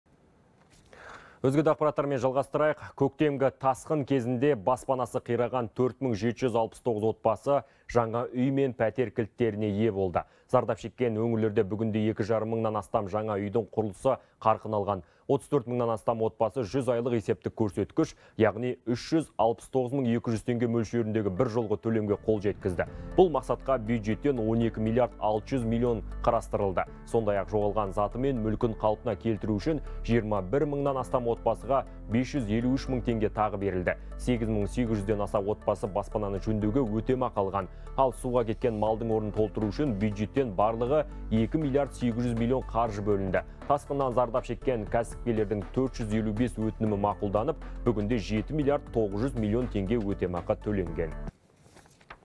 Көктемгі тасқын: 4769 отбасы жаңа үй кілттеріне ие болды
Көктемгі тасқын кезінде баспанасы қираған 4769 отбасы жаңа үй мен пәтер кілттеріне ие болды, деп хабарлайды 24kz.